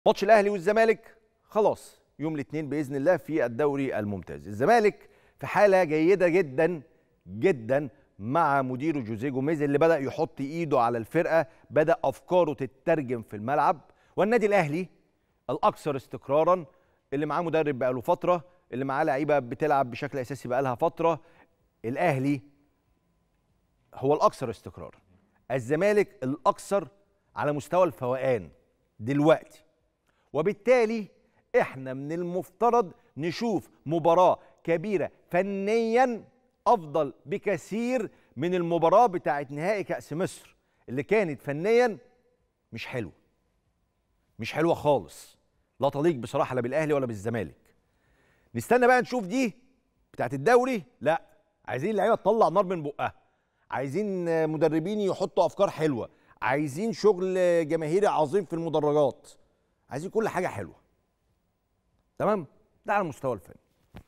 "عايزين اللاعيبة تطلع نار من بوقها"🔴⚪️مقدمة استثنائية من كريم رمزي عن مباراة الأهلي والزمالك⚽️🏆
"عايزين اللاعيبة تطلع نار من بوقها"🔴⚪️مقدمة استثنائية من كريم رمزي عن مباراة القمة يوم الاثنين بين الأهلي والزمالك بدوري نايل ⚽️🏆